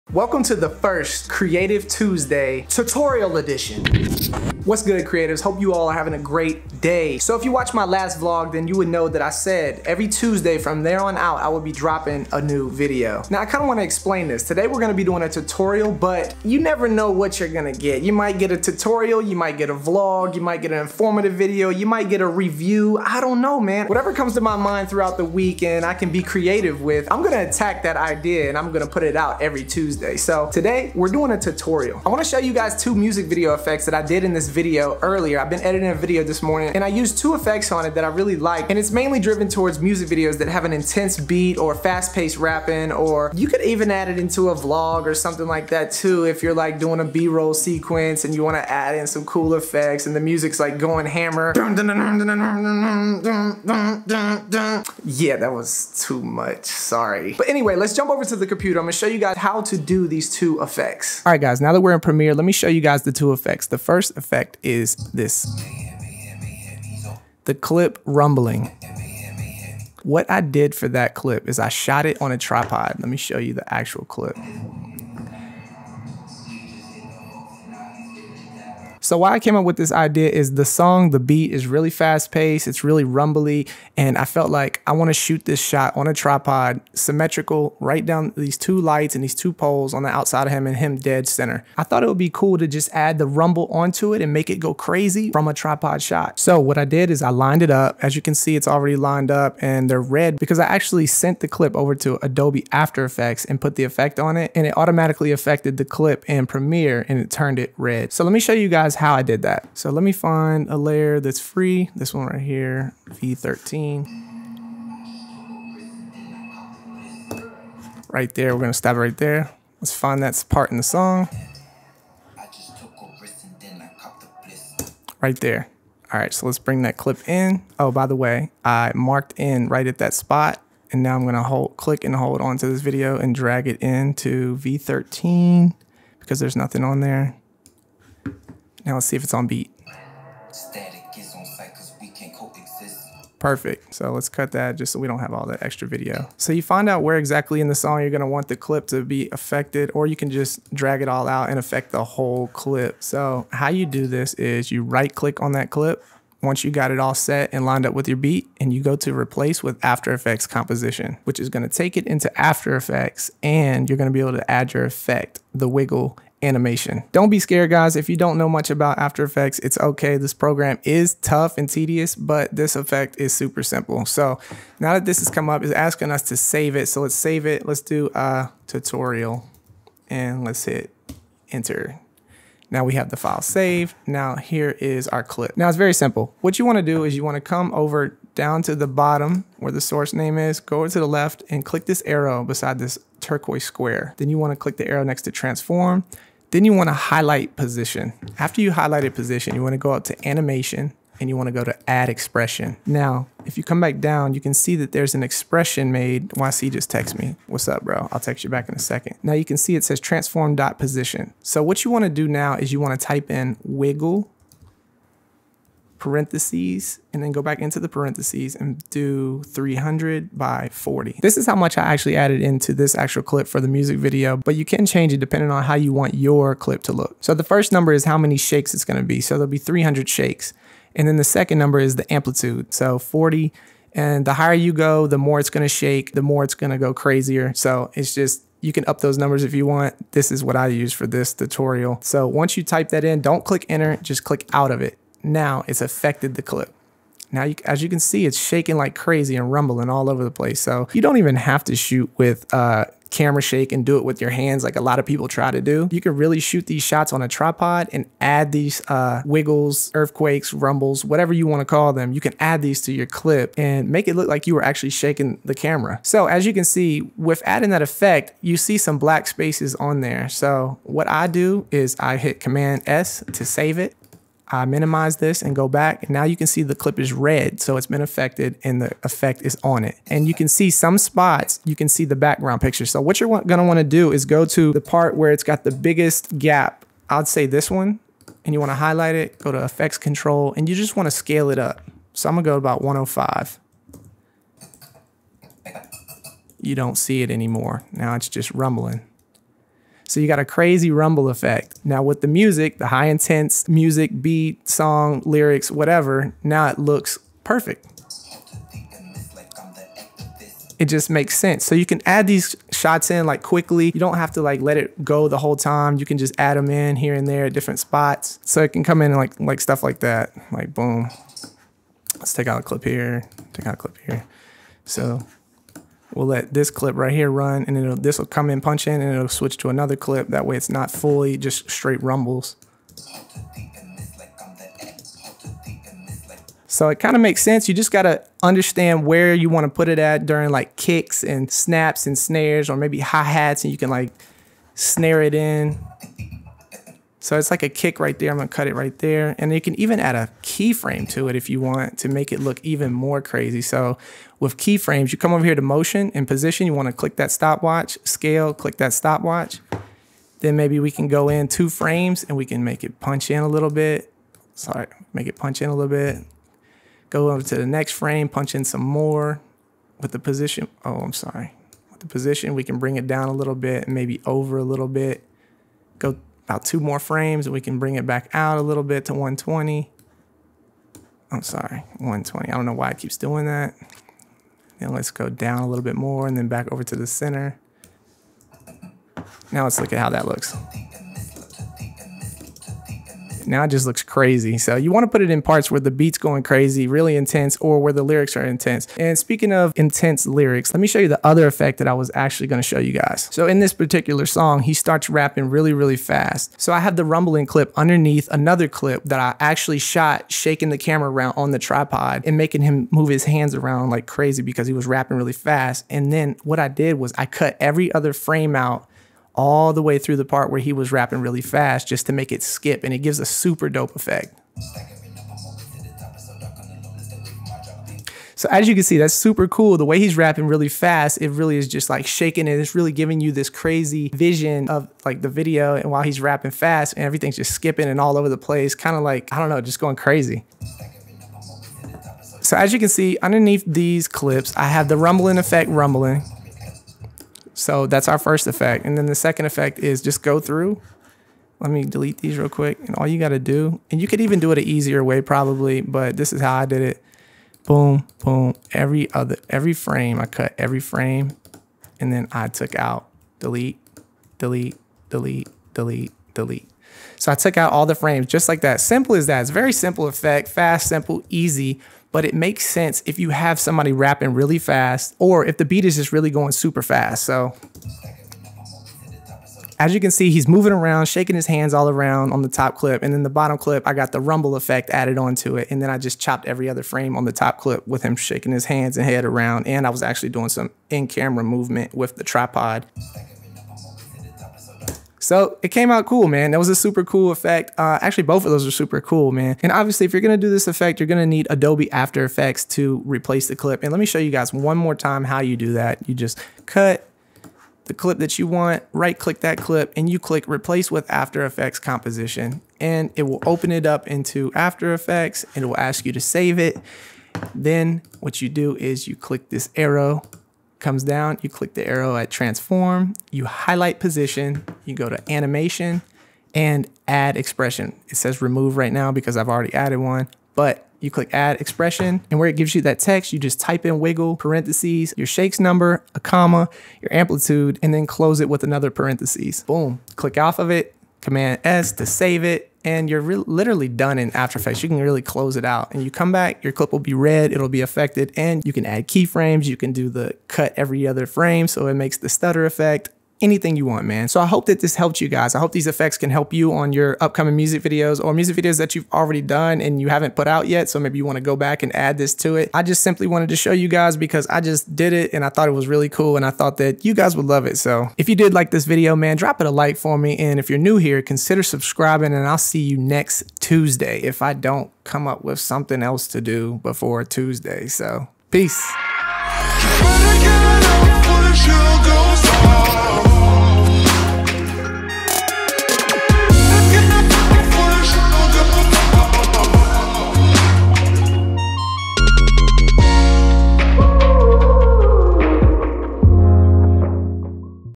2 Music Video Effects Tutorial (NO PLUGINS)
In this tutorial, I show you how to do 2 music video effects in Adobe Premiere Pro & Adobe After Effects. Rumble & long paced stuttering effects.

You can use these effects on ANY type of video project, not just music videos and they are EASY to create! You should be able to recreate these in other editing programs as well!

Music (get 30-days FREE using this link!)

Gear Used Here:
1DX Mark ii
Canon 17-40mm
Sigma Art 24mm